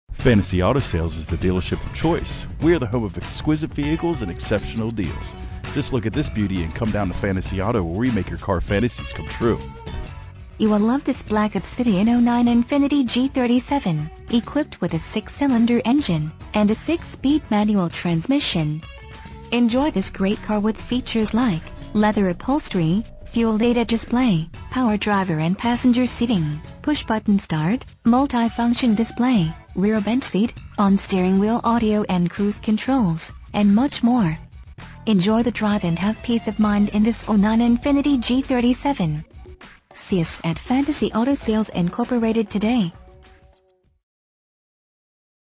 2009 Infiniti G37 Sedan Fantasy Auto Sales Inc Phoenix, AZ 85020
Infiniti G Sedan Imagine driving this  Black Obsidian   Infiniti G37 Sedan 4 Dr Sedan, equipped with a 6 Cylinder engine and a 6-speed manual transmission. Enjoy this great car with features like Leather Upholstery, Fuel Data Display, Power Driver and Passenger Seating, Push Button Start, Multifunction Display, Rear Bench Seat, on steering wheel audio and cruise controls, and much more. Enjoy the drive and have peace of mind in this  Infiniti G37 Sedan. See us at Fantasy Auto Sales Inc today!
MILES: 50,717
VIN:JNKCV61EX9M301178

9102 N. 7th Street